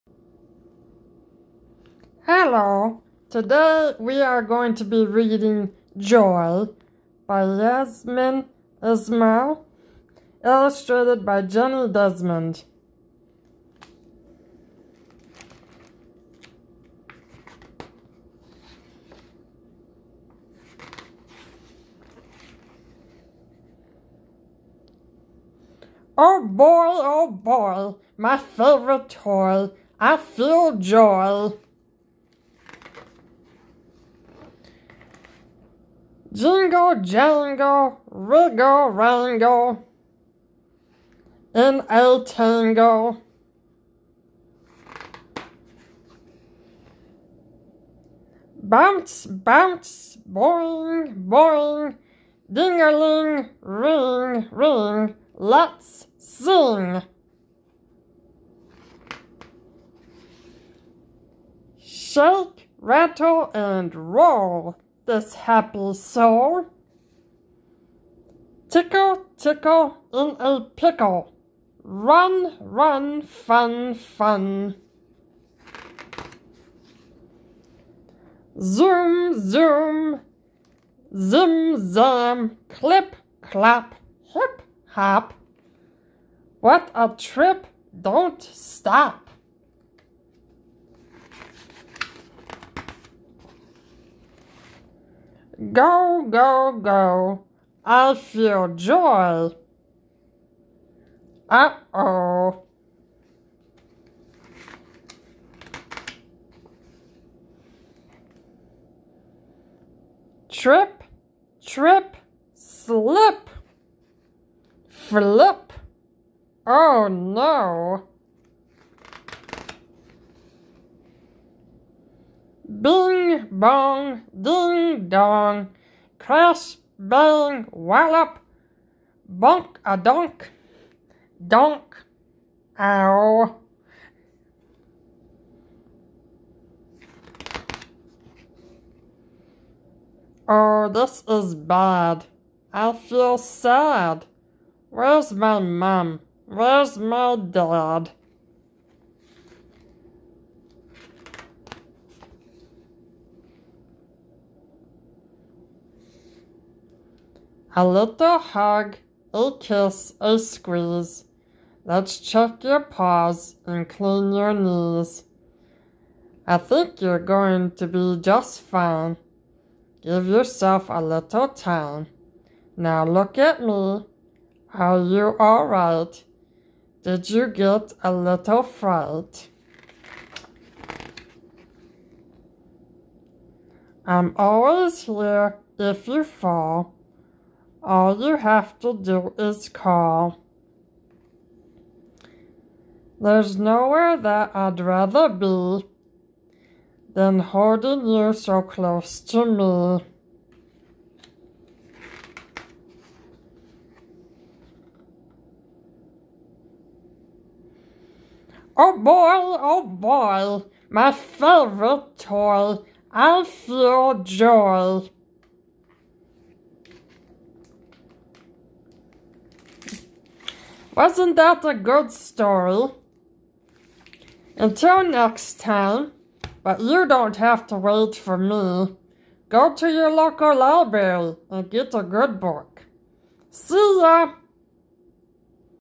Joy by Yasmeen Ismail Read Along
A cute story about a playful kitten read in an interesting voice.